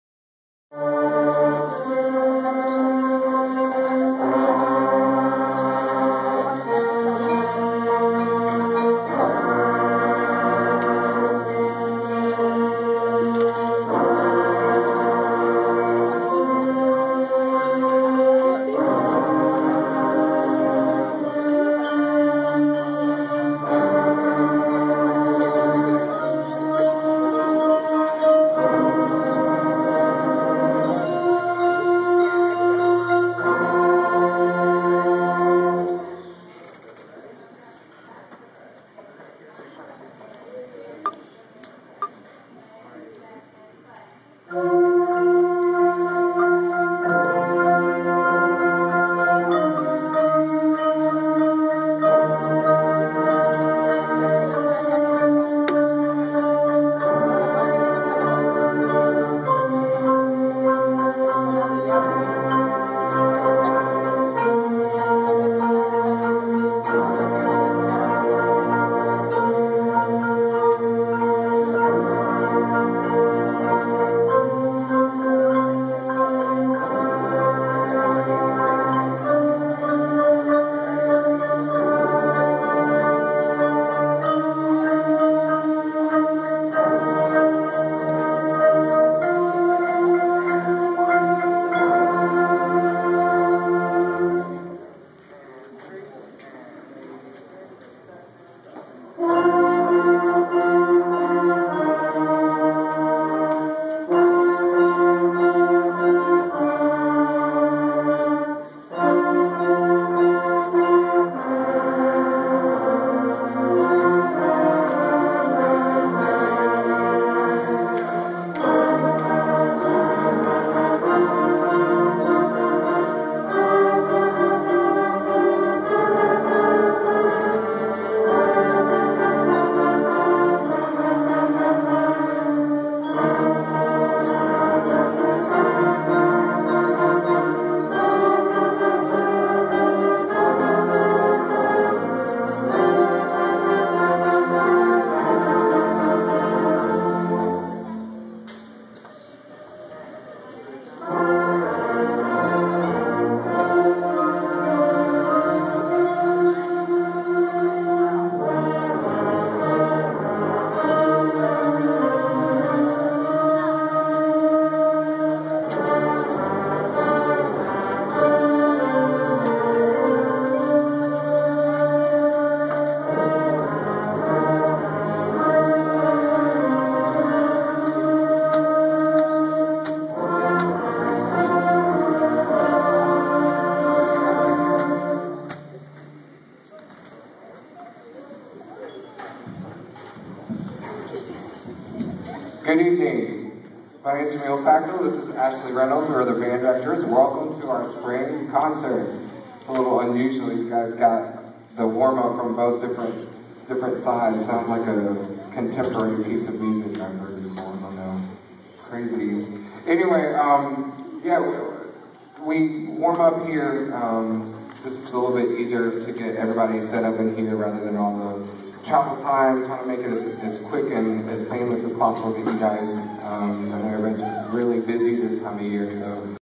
Megan's 6th grade band recital part 1
My Daughter's "Megan Moon" 6th grade Band Recital. She is in the first row playing in the Flute Section with her hair in a pony tail. Way to Go Megan! Daddy's Girl Woo Woo!...Hoot Hoot! :-). I'm so proud of her